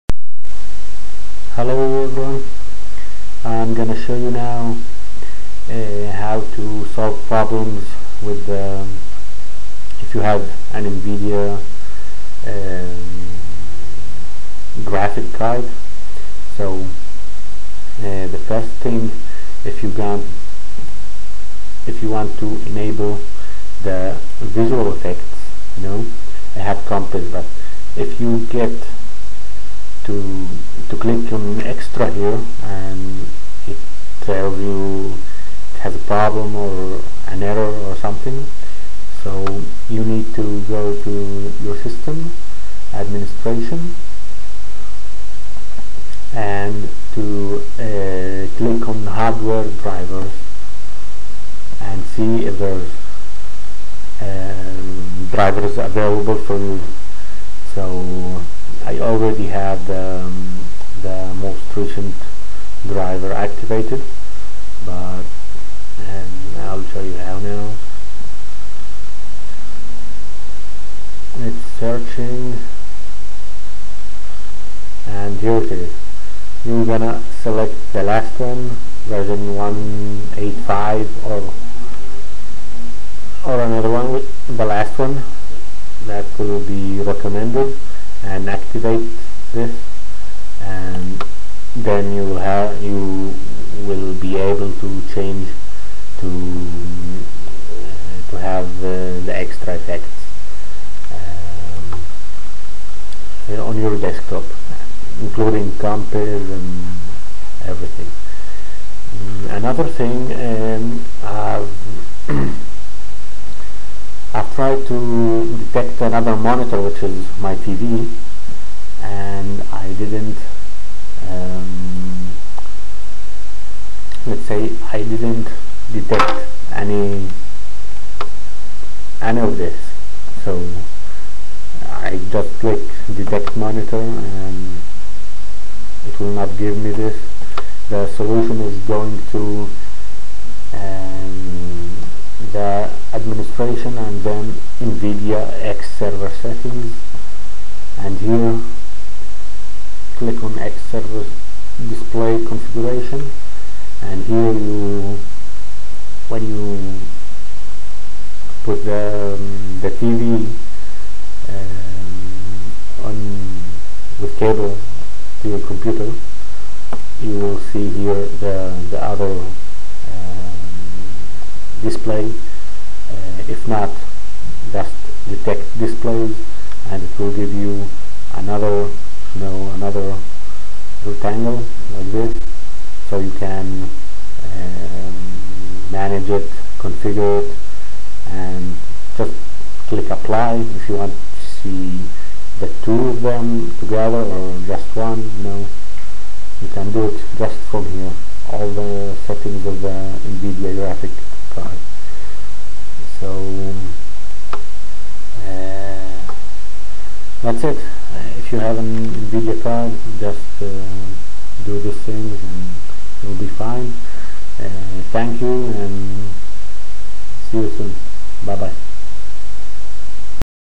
ubuntu - nvidia, howto detect a screen and enable extra effects
how to enable the extra ubuntu effects and detect other monitors!